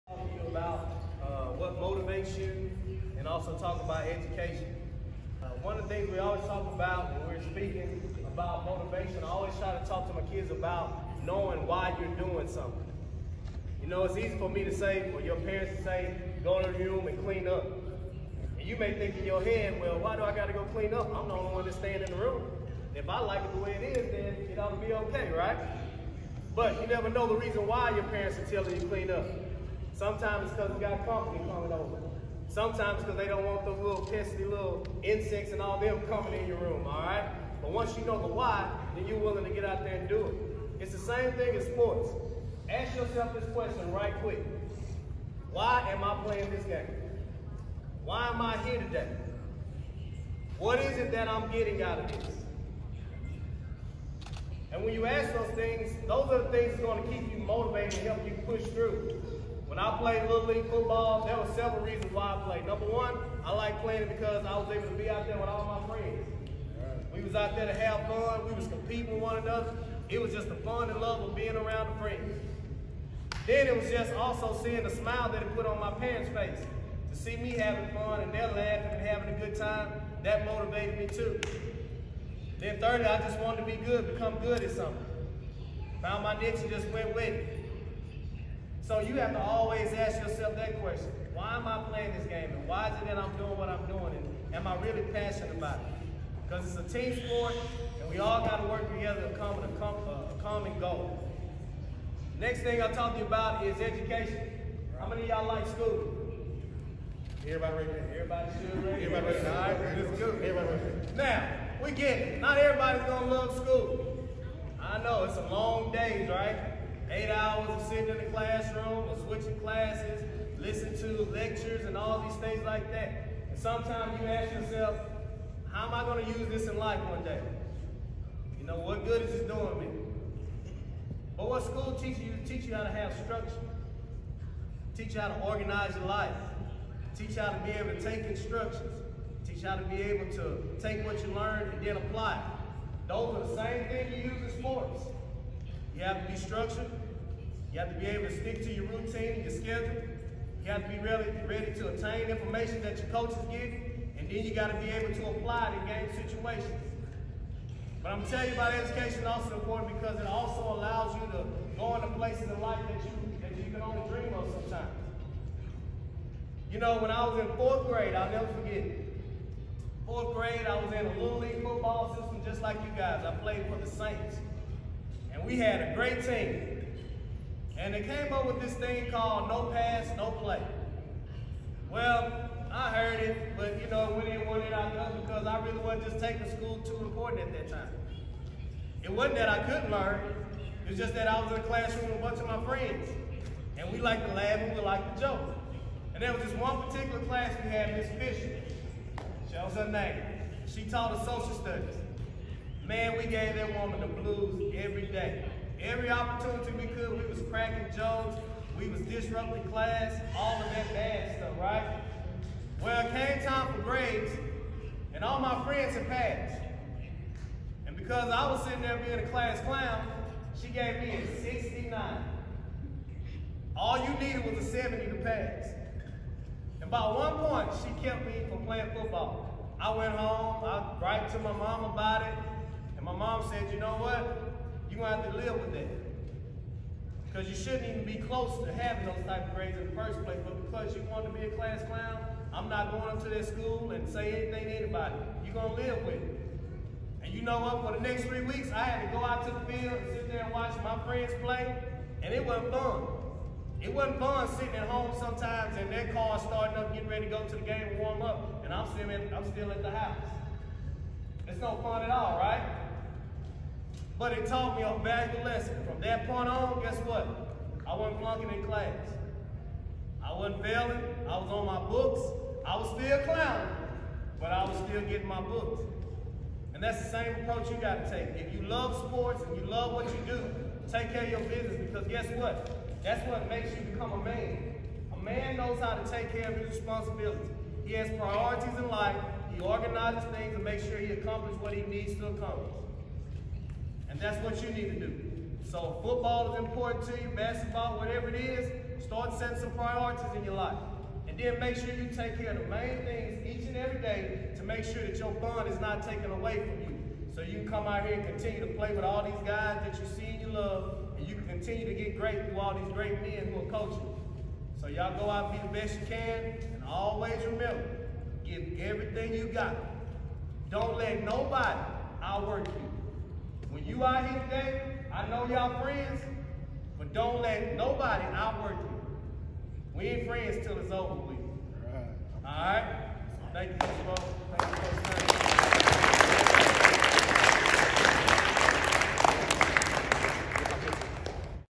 LaMarcus Franklin speaks to a group of kids
Great speech from a football coach